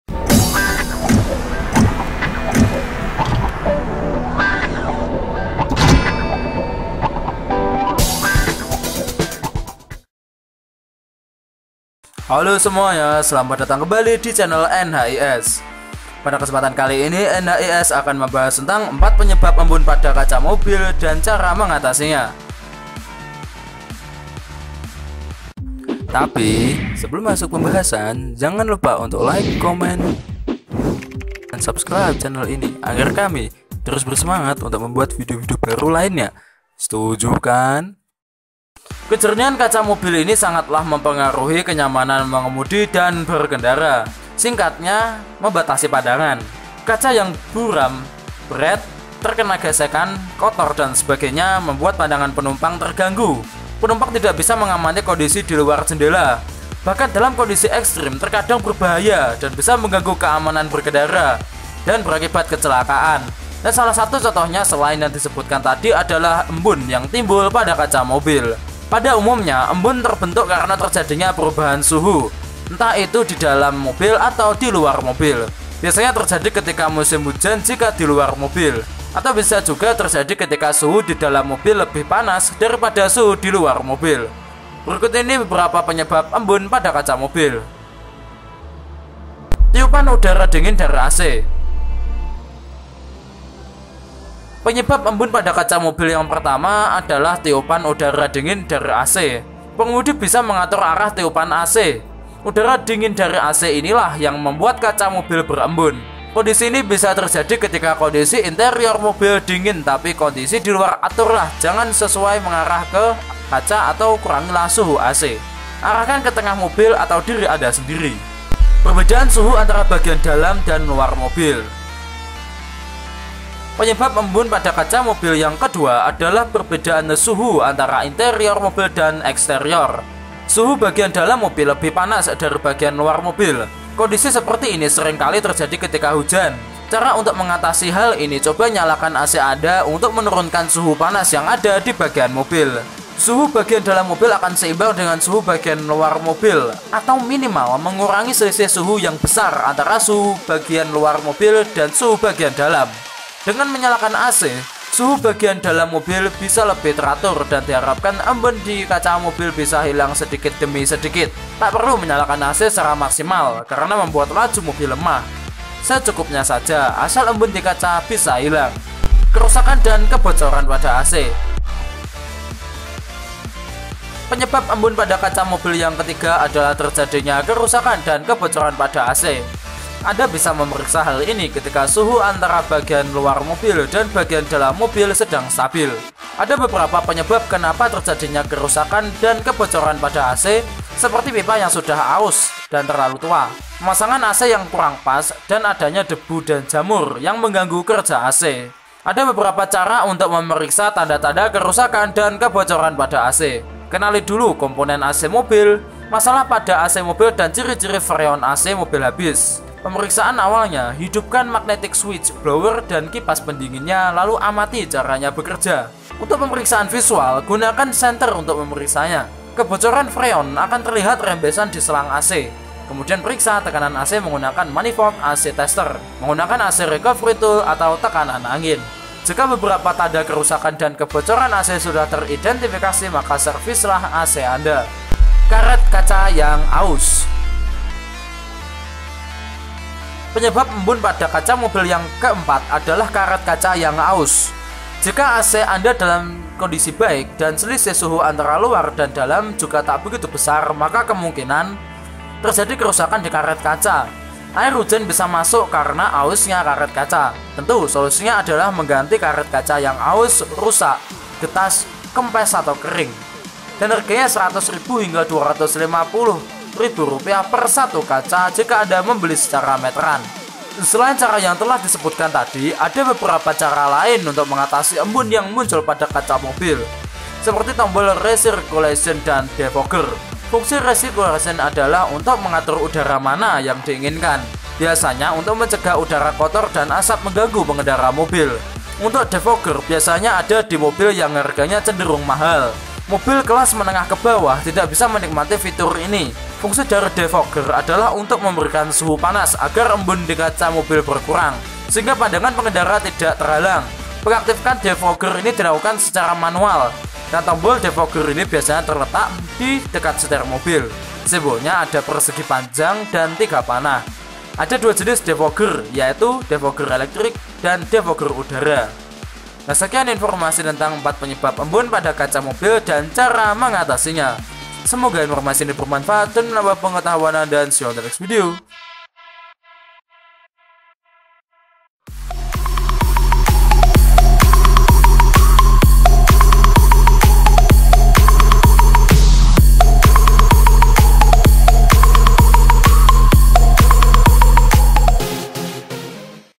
4 Penyebab Embun Pada Kaca Mobil dan Cara Mengatasinya
Hello guys, kali ini kami akan menyajikan video tentang 4 Penyebab Embun Pada Kaca Mobil dan Cara Mengatasinya.